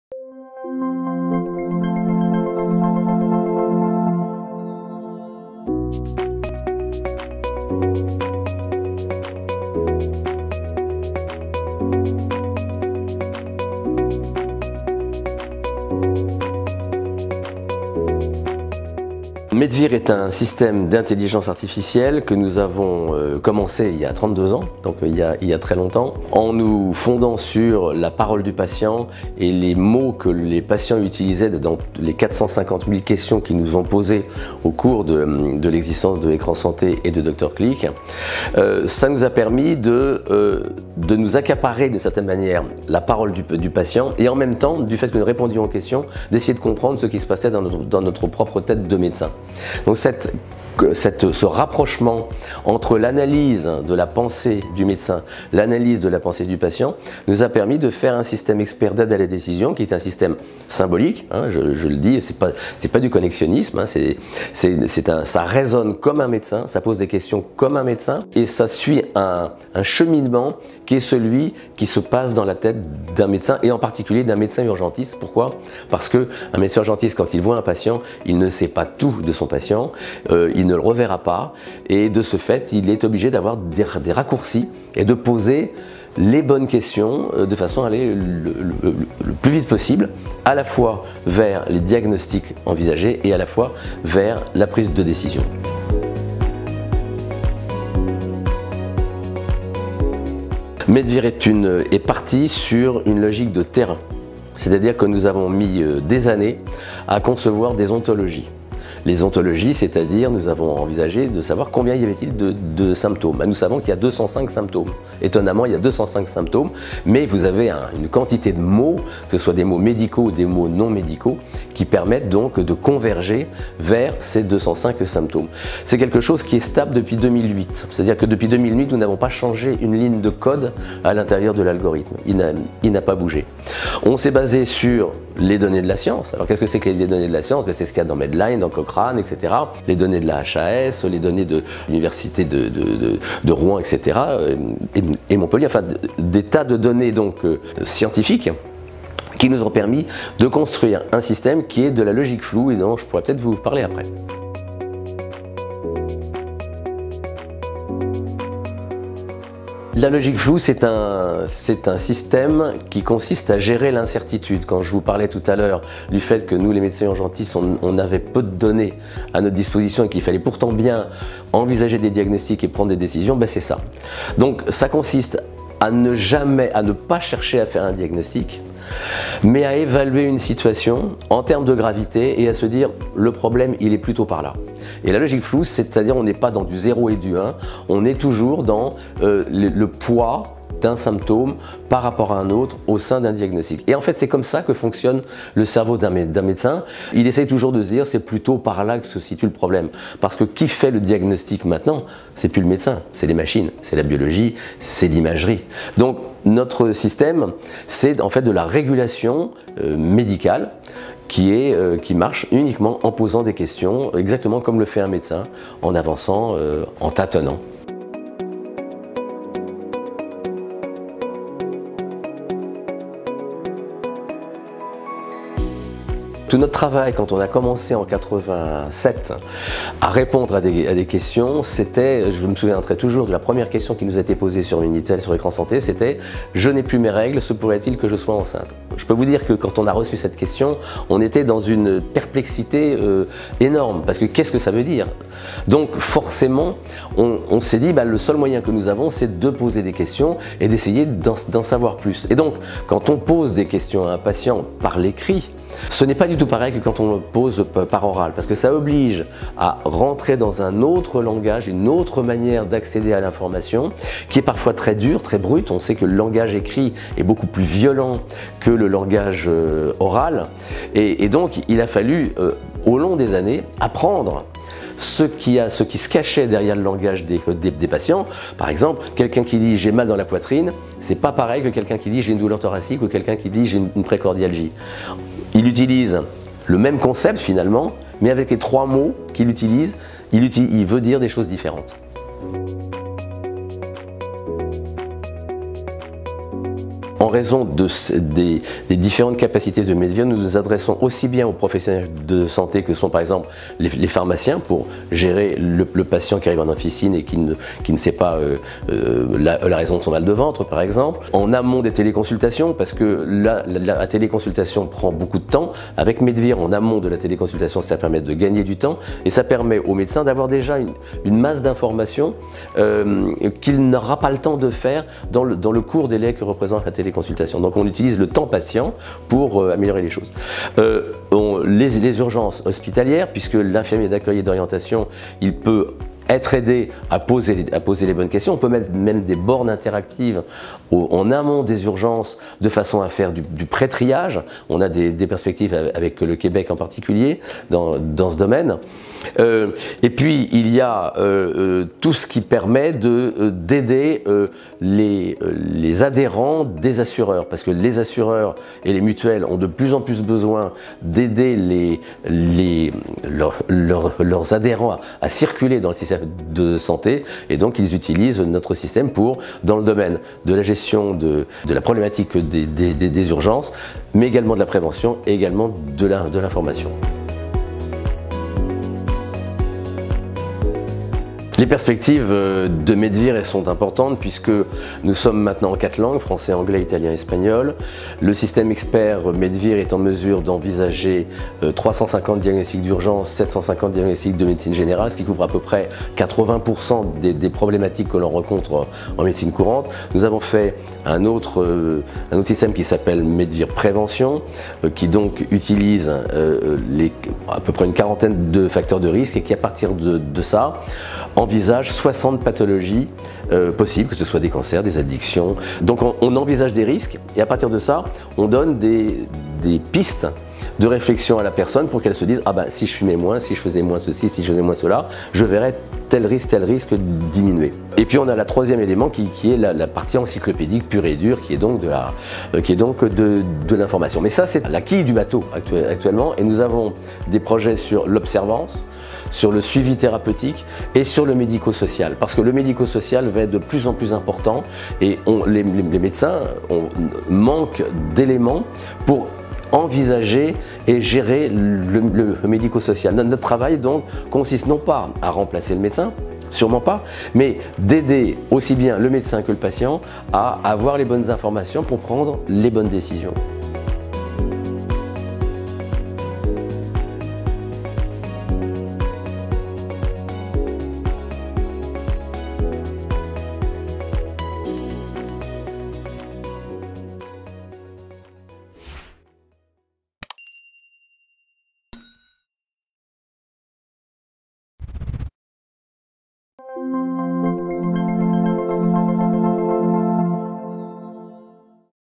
MEDVIR, l’intelligence artificielle au service de la gestion des urgences (Start-up Coup de Coeur)
Beesens vous propose de découvrir MEDVIR (notre Start-up COUP DE CŒUR en juin 2020) qui dispose d’une solution d’intelligence artificielle pour accompagner les professionnels de santé dans leurs prises de décision.
Loïc ETIENNE, médecin urgentiste est CEO de Medical Intelligence Services qui porte la solution MEDVIR.
Il nous présente sa start-up en plein développement.